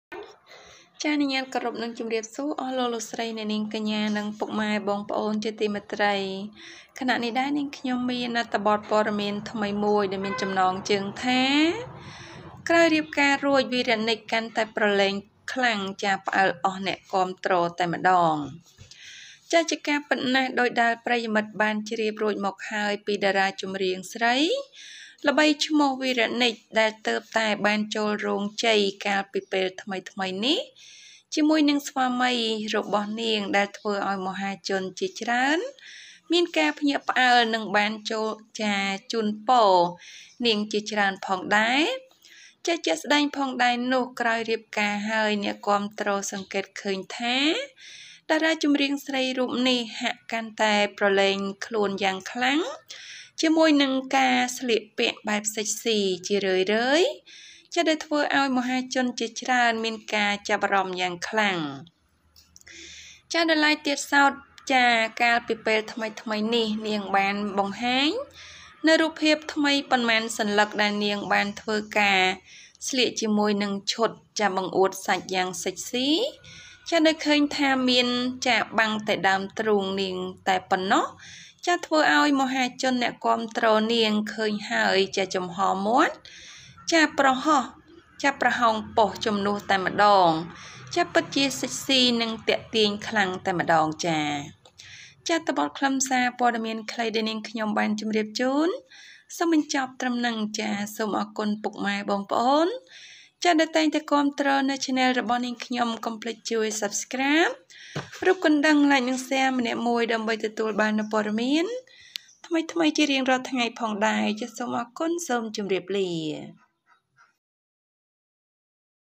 After getting married, Vireaknich became more and more playful, surprising all the fans
ក្រោយរៀបការរួច វីរៈនិច្ច កាន់តែប្រលែងខ្លាំង ផ្អើលអស់អ្នកគាំទ្រតែម្ដង